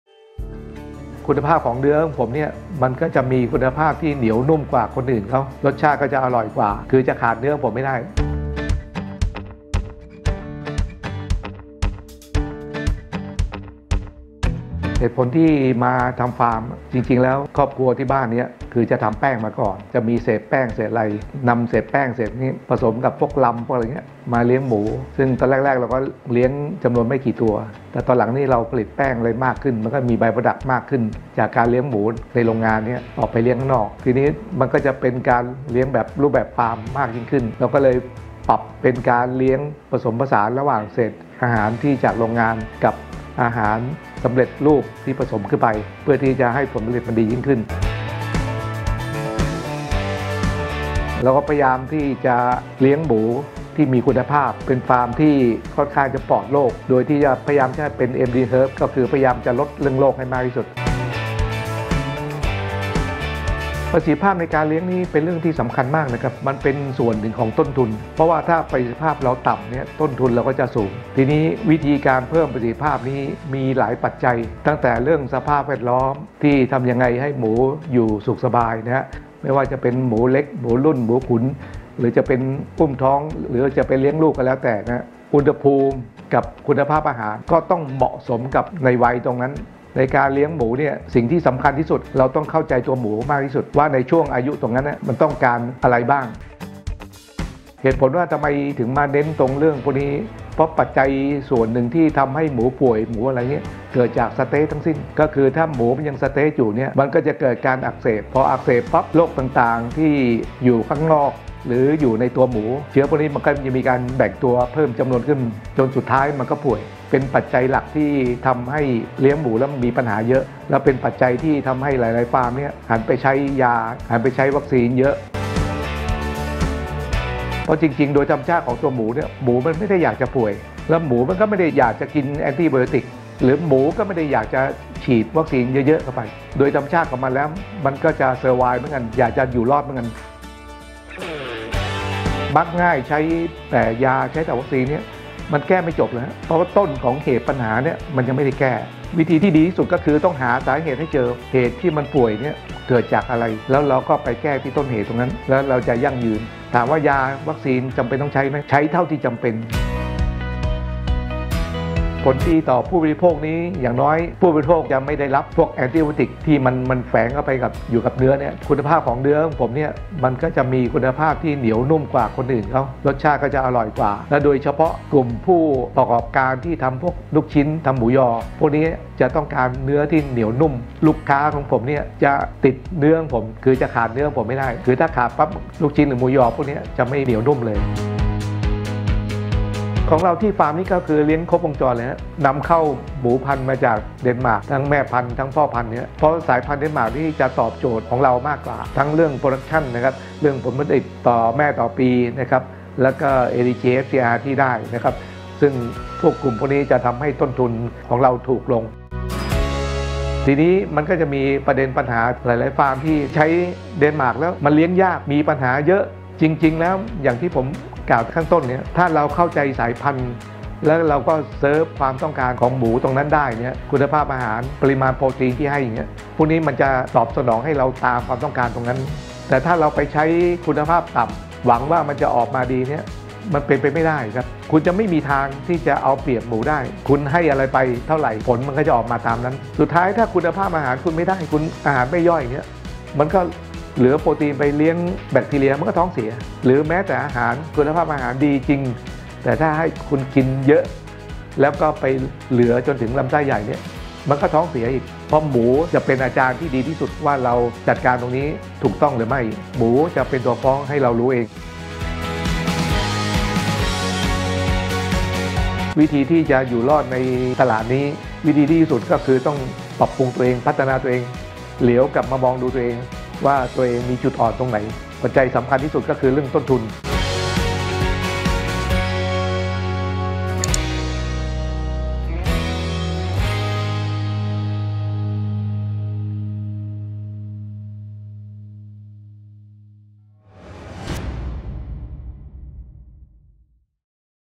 ฟาร์มพะเยาอะโกรเทค ฟาร์มหมูท้องถิ่น มัดใจคนพะเยาด้วยคุณภาพ
คุณนิวัฒน์ ฤทธิ์วิวัฒน์ (โกคิ้ม) เจ้าของ "ฟาร์มพะเยาอะโกรเทค" ต.แม่ต๋ำ อ.เมืองพะเยา จ.พะเยา ผู้ที่เลี้ยงหมูโดยเน้นให้หมูมีคุณภาพ ปลอดโรค เพิ่มประสิทธิภาพการเลี้ยง เพื่อให้ต้นทุนการผลิตต่ำลง ที่สำคัญเป็นที่ต้องการของตลาด และยั่งยืน

ติดตามช่องทางอื่นของเราได้ที่นี่